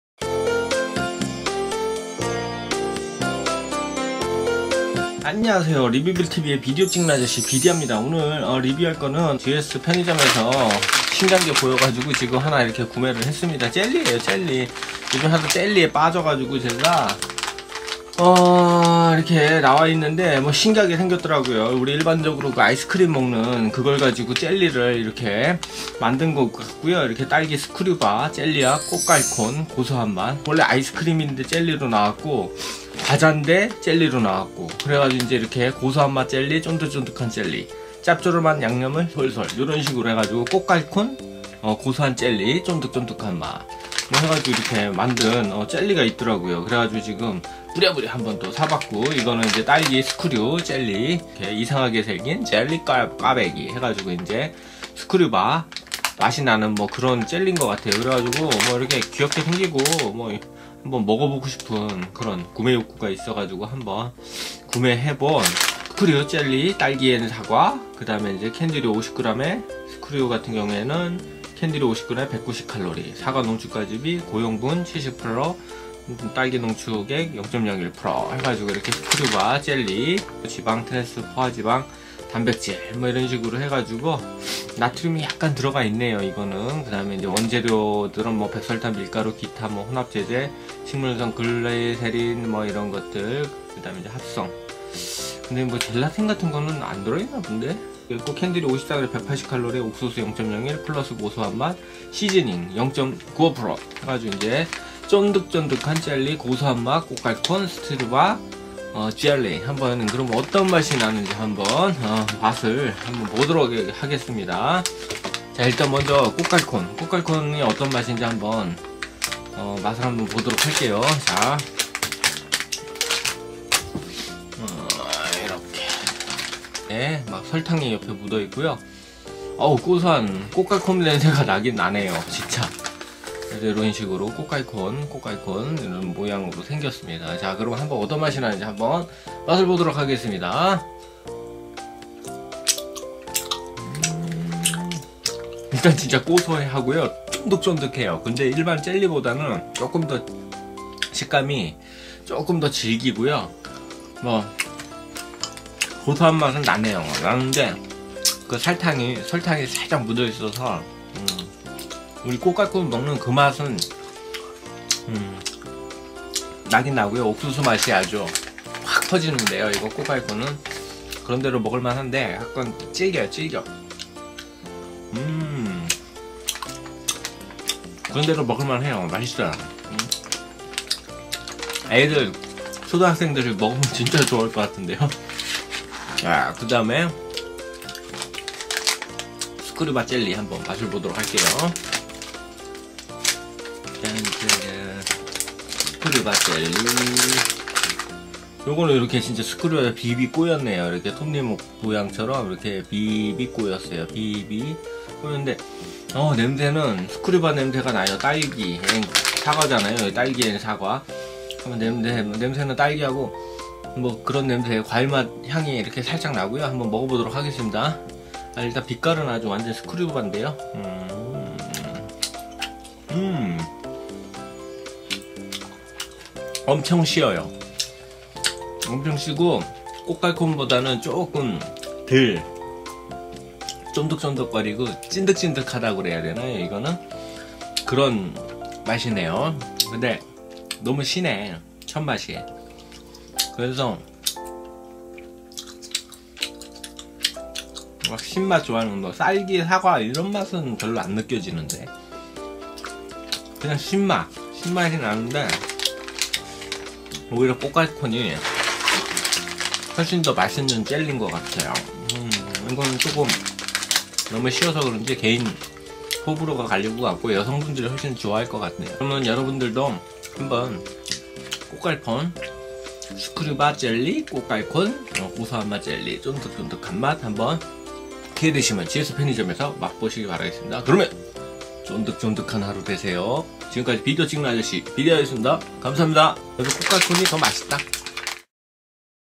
롯데제과가 꼬깔콘젤리와 스크류바 젤리를세븐일레븐과 GS25 그리고 CU와 콜라보한 젤리 맛은?
스크류바 아이스크림을 딸기와 사과맛 젤리로 재탄생 시키고
꼬깔콘은 고소한맛 젤리로 과자의 맛과 재미로 이색적으로 내놓은
젤리 콤비네이션 익숙한 다양한 제품을 젤리로 맛보는 재미가 매력 있는 맛으로 다가오네용~ 스크류바젤리는 강한 새콤한 맛이라면
꼬깔콘은 호불호가 나뉠만한 맛이긴 하나 개인적으론 괜찮은 맛이였답니다. ^^::